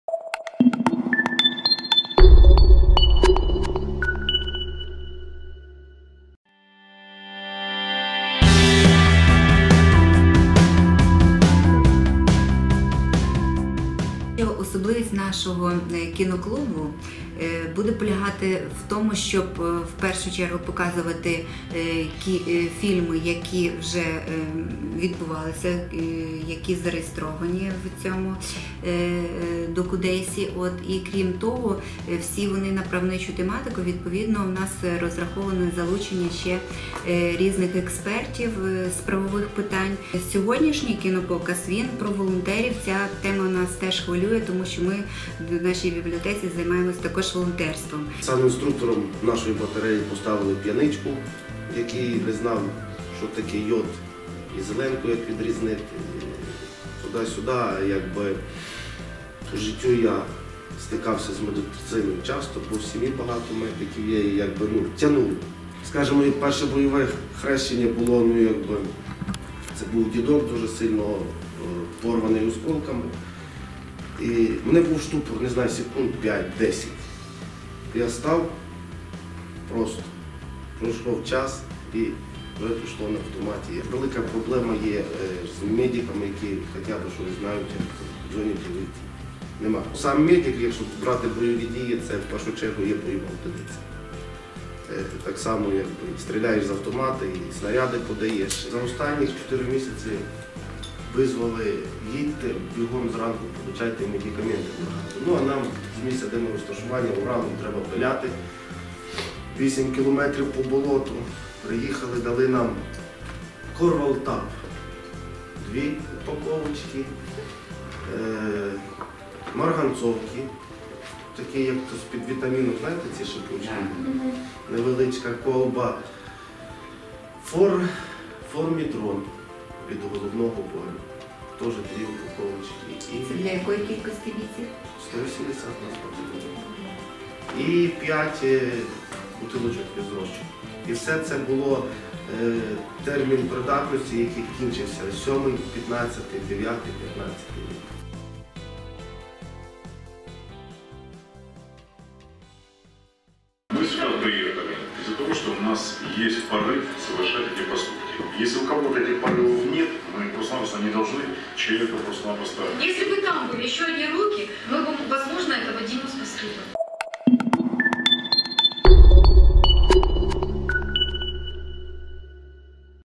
У Луцьку відкрили правовий кіноклуб
У Волинська обласна бібліотека для юнацтва розпочав свою роботу кіноклуб Docudays UA «На Волі 2». Вже декілька років вони співпрацюють із Міжнародним мандрівним фестивалем правового кіно. Тепер кінопокази у бібліотеці стануть регулярними.
Керівник кіноклубу Любов Радинська зазначила:
«Особливість нашого кіноклубу буде полягати в тому, щоб показувати фільми, які вже демонструвалися на фестивалі. Усі вони на правничу тематику. Відповідно, ми розраховуємо залучати до співпраці різних експертів з правових питань».
Перший кінопоказ присвятили темі волонтерів-лікарів в зоні АТО. Цього разу запросили волонтера, бійця  14-ої окремої механізованої бригади, санінструктора протитанкового дивізіону Тарас Самчук із позивним «Цитрамон». Свого часу, хлопець проходив військову службу у Білій церкві (71-бригада). Після початку анексії Криму пішов у військомат. Призвали його у 51-у бригаду, де «поставили номером обслуги». Проте з часом довелося перейти у медики.
«Велика проблема є з медиками, які хоча б щось знають і якось орієнтуються, - розповів Тарас. -  Сам медик під час бойових дій - це, в першу чергу, бойова одиниця: так само стріляєш з автомата, снаряди подаєш...»
Першим фільмом, який подивилися відвідувачі кіноклуб «На Волі 2» став «Хоттабич і його команда».